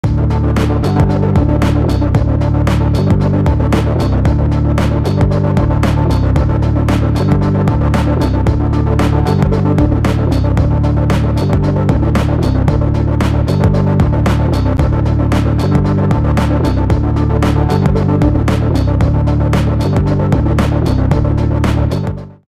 Exploring a world made in 10 minutes!
In last video, we showed you how to build a scene in 10 min.
Now we are going to explore it!
YAHAHA Studio provides excellent 3D content creating tools.

Some of the art assets in the video are purchased and used only for concept demonstration purposes.